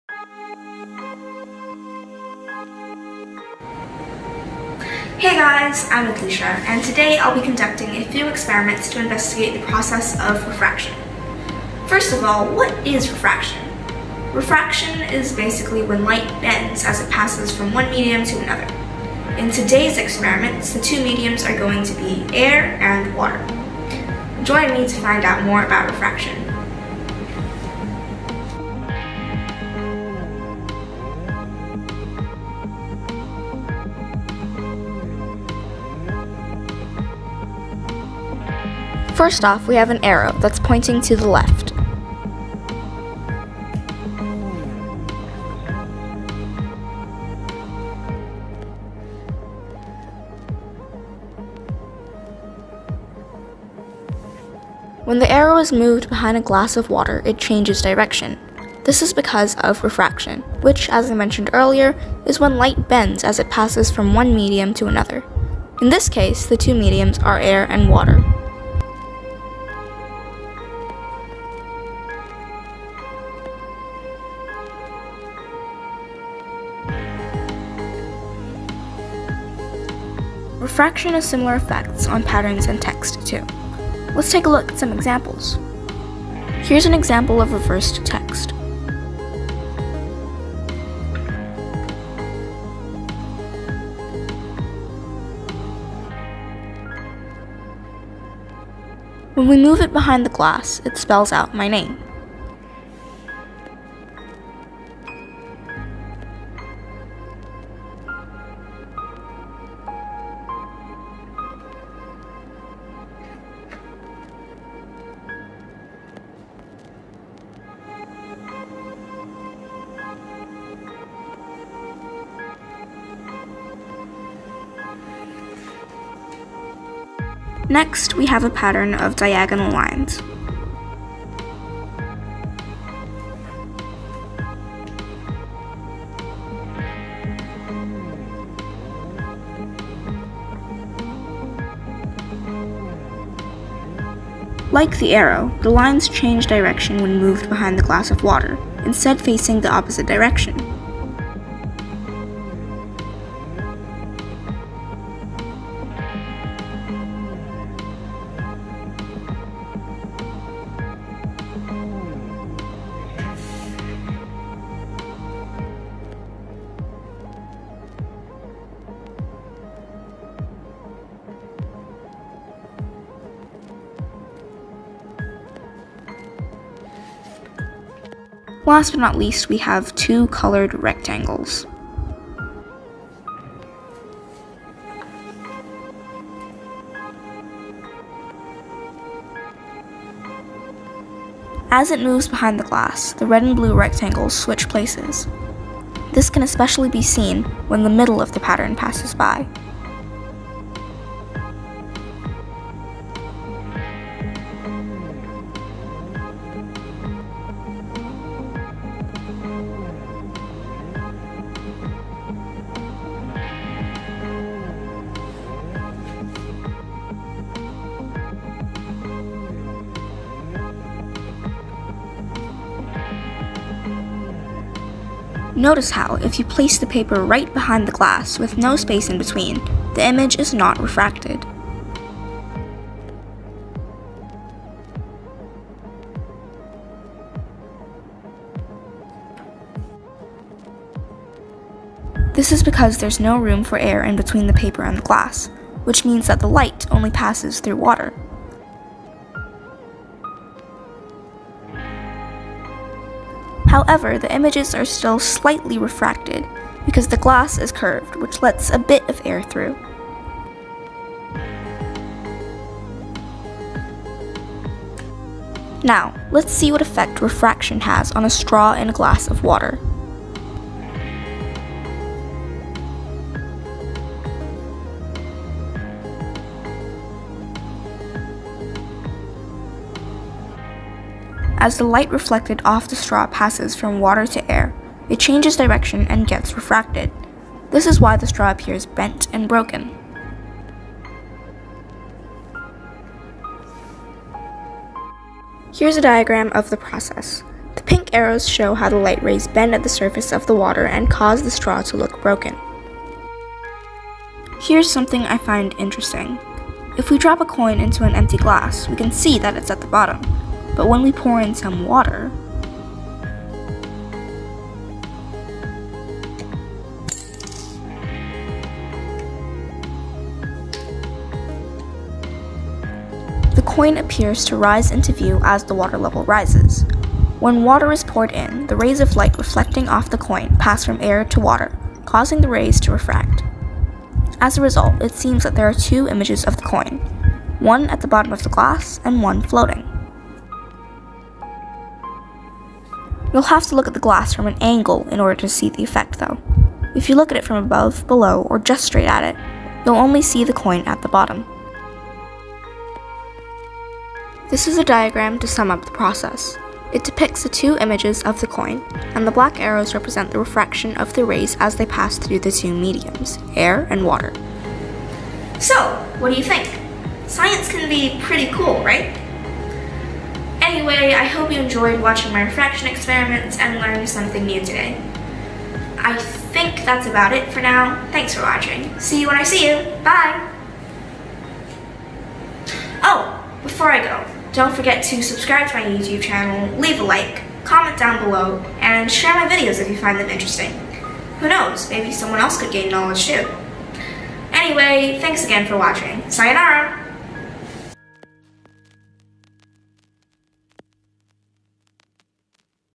KS3 Y8 Science - Light Refraction by Aedlycia
Hi, everyone! I'm Aedlycia​ - 11 y.o. I'm currently in Secondary Year 8 in one of the international schools in Malaysia. As the world continues to observe the fluctuation of​ Covid19Pandemic cases​, Malaysia is still closing its international border, and my school has closed from time to time depending on the local community cases, but learning is still going on - either physically or online (DistanceLearning​) - and students have been instructed by their respective teachers to complete the given tasks at home. So here's one of my Science tasks, where I'll need to explain the above mentioned topic.